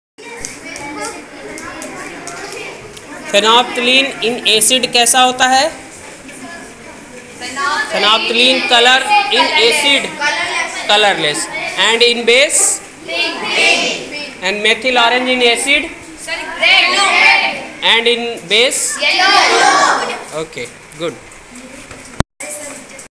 Indicators
chemistry indicators in acid and base changes their colour.